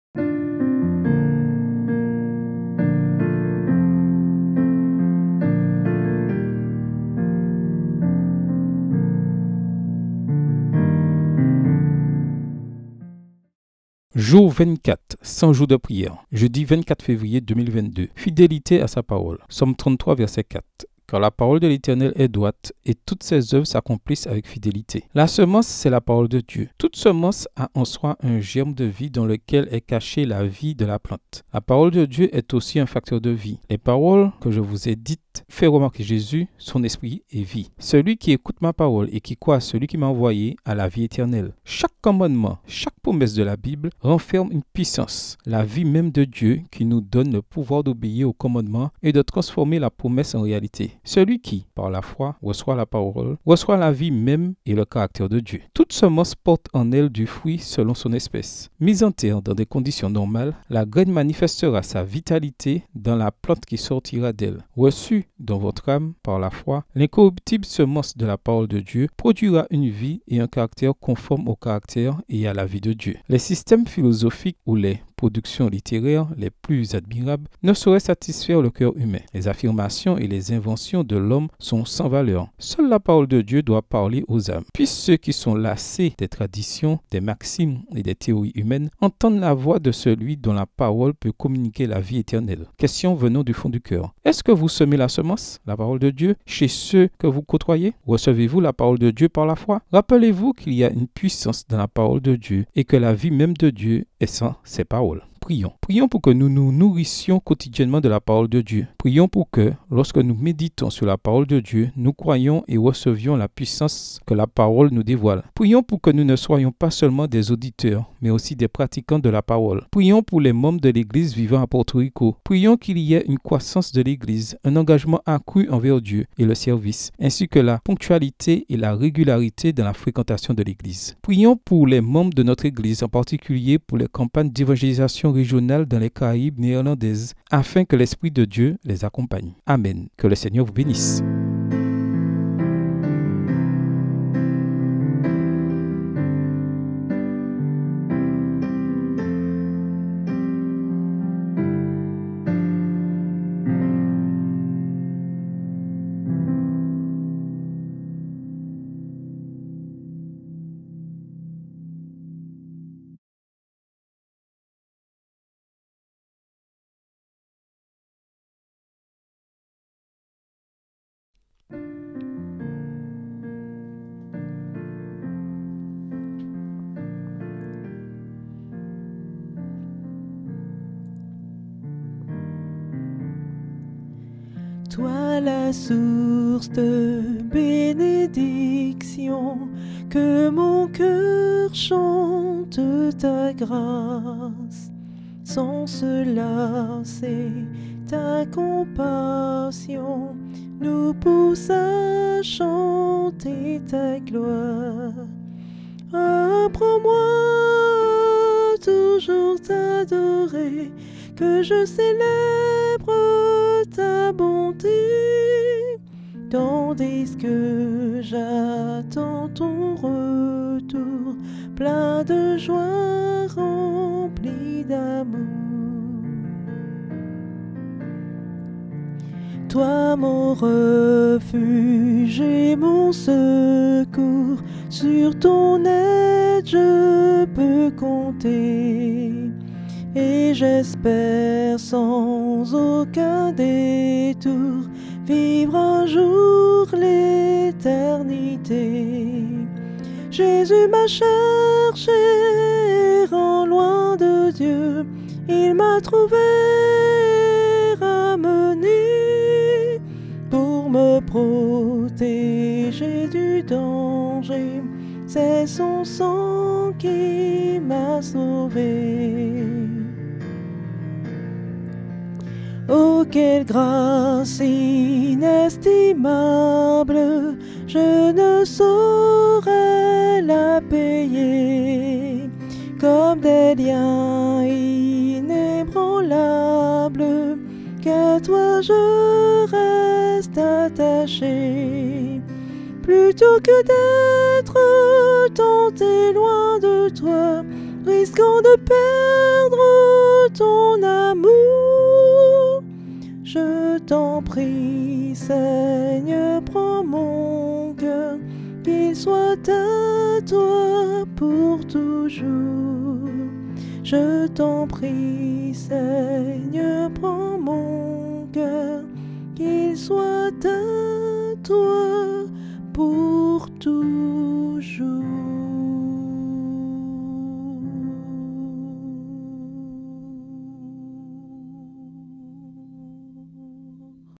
Jour 52 – Jeudi 24 mars 2022 – Un cœur abandonné à Dieu
Cliquez sur le lien ci-dessous pour écouter Jour 52 100 jours de prière

C’est de là aussi que tu chercheras l’Éternel, ton Dieu, et que tu le trouveras, si tu le cherches de tout ton cœur et de toute ton âme. – Deutéronome 4:29
Chaque individu doit chercher par une prière sincère à connaître la Parole de Dieu par lui-même, puis à la mettre en pratique. Ce n’est qu’en mettant jour après jour sa confiance en Dieu, et non dans le bras de la chair, qu’une âme obtiendra l’expérience essentielle pour répondre à la prière du Christ : « Or, la vie éternelle, c’est qu’ils te connaissent, toi, le seul vrai Dieu, et Jésus-Christ, que tu as envoyé » (Jean 17:3). Telle est la leçon donnée à toute âme qui a commencé la nouvelle année. Dans toutes vos préoccupations temporelles, dans tous vos soucis et toutes vos angoisses, attendez-vous au Seigneur. Ne mettez pas votre confiance dans les princes, ni dans les fils de l’homme parce qu’ils peuvent être dans des positions de confiance. Le Seigneur a uni votre cœur à Lui. Si vous L’aimez, et si vous êtes accepté à Son service, apportez tous vos fardeaux, tant publics que privés, au Seigneur et espérez en Lui. Vous aurez alors une expérience individuelle, une conviction de Sa présence et de Sa disponibilité à entendre votre prière pour la sagesse et l’instruction qui vous donnera l’assurance et la confiance dans la volonté du Seigneur de vous secourir dans vos perplexités. {TDG 82.3}
Questions au cœur : Recherchez-vous le Seigneur votre Dieu ? Priez-vous sincèrement pour connaître la Parole de Dieu ? Mettez-vous quotidiennement votre confiance en Dieu et non dans le bras de la chair ? Apportez-vous vos fardeaux, tant publics que privés, au Seigneur et vous attendez-vous à Lui ?
1. Priez pour que vous recherchiez Dieu de tout votre cœur et de toute votre âme afin d’avoir un cœur qui s’abandonne.
2. Priez pour les membres de l’église vivant à Antigua. Priez pour qu’il y ait une croissance de l’église, un engagement accru envers Dieu et le service, ainsi qu’une ponctualité et une constance dans l’assistance.
3. Priez pour une vision céleste afin que nous puissions voir au-delà de la tombe, au-delà de la douleur, de la souffrance et des difficultés de cette vie, pour voir la récompense céleste qui attend ceux qui sont fidèles jusqu’à la fin.
4. Priez pour que les anciens de nos églises, alors qu’ils sont instruits des doctrines et des principes fondamentaux, permettent au Saint-Esprit de diriger leur vie.